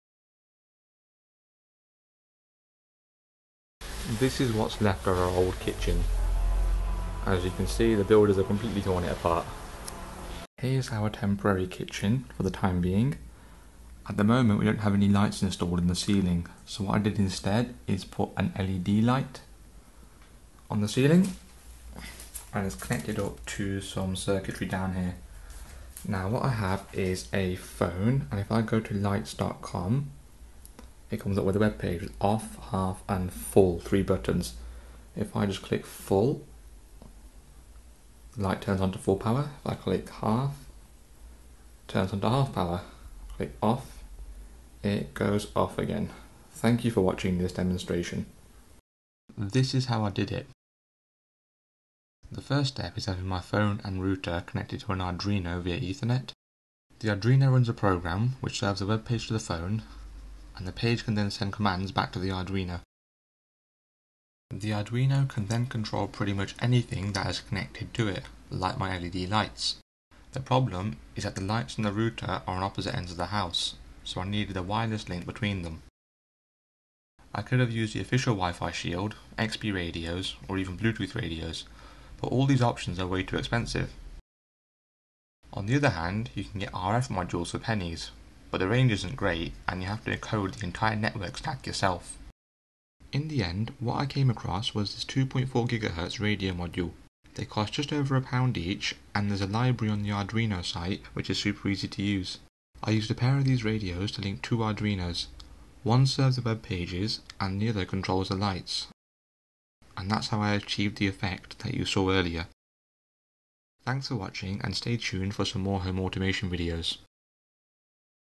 Smartphone Controlled LED Lights
This is a system I built that allows me to control LED lights from my phone.